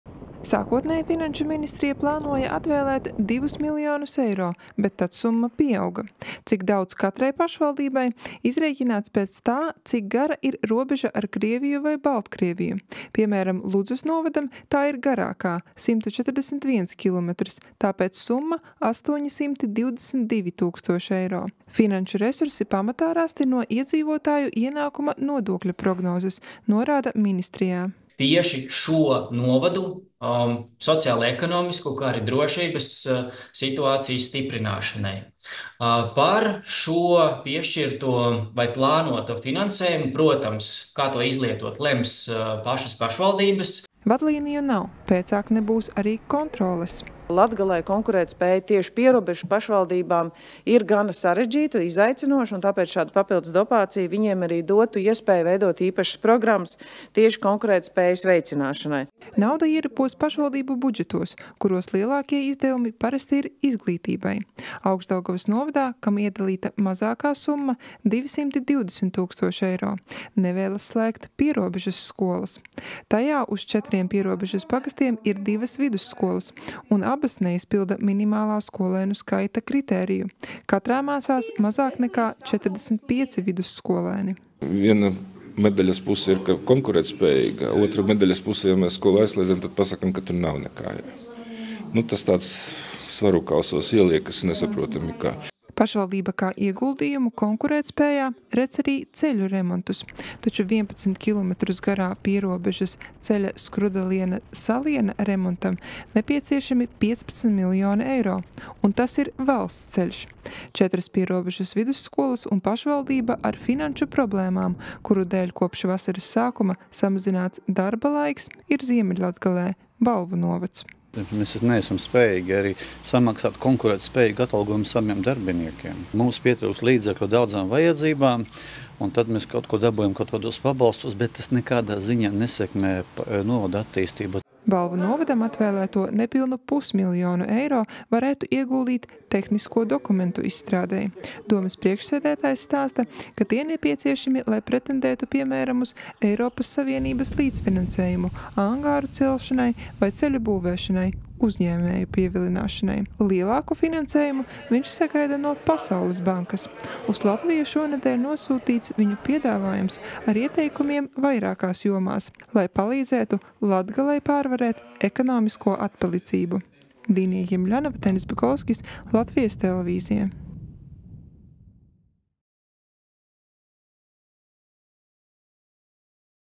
Konkurētspējai pierobežā atvēl 2,5 miljonus eiro
Šonedēļ valdības un pašvaldību panāktā vienošanās paredz 2,5 miljonus eiro speciālā dotācijā atvēlēt pieciem novadiem ar ārējo Eiropas Savienības robežu. Cik katrai pienākas, rēķinās pēc robežas garuma. Šāda dotācija tiek piešķirta pirmo reizi, atzīt pašvaldību vadītāji. Premjere iesaka stiprināt konkurētspēju, bet noteikumu, kam tieši naudu veltīt nav. Vietvaras pašas lems, ķerties klāt uzņēmēju meklēšanai vai skolu glābšanai.